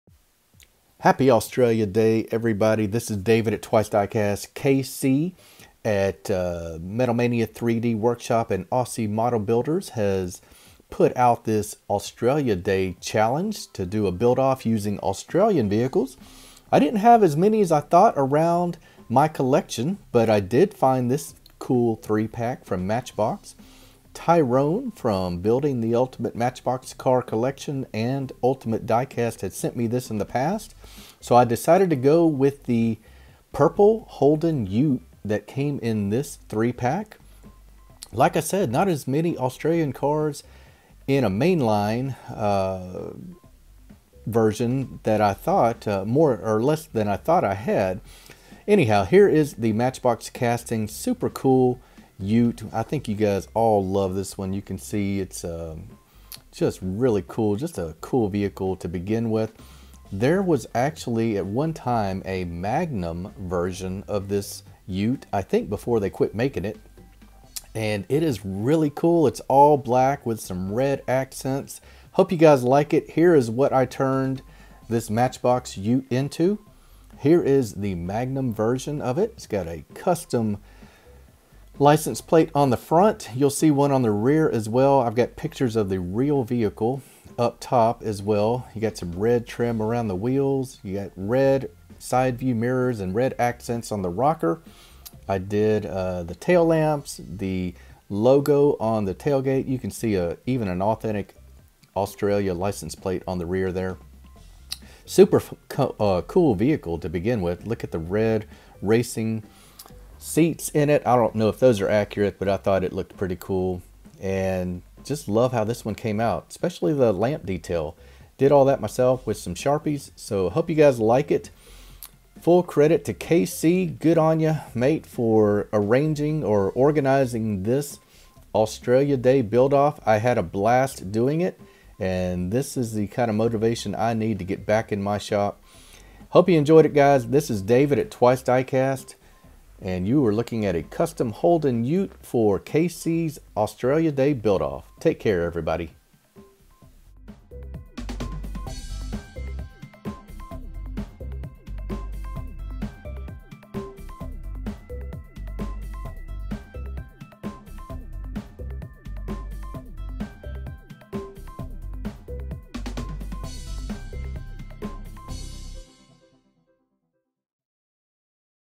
Australia Day Build - Thanks to KC at Aussie Model Builders
KC challenged the diecast customizers to build something Aussie for Australia Day, have a look!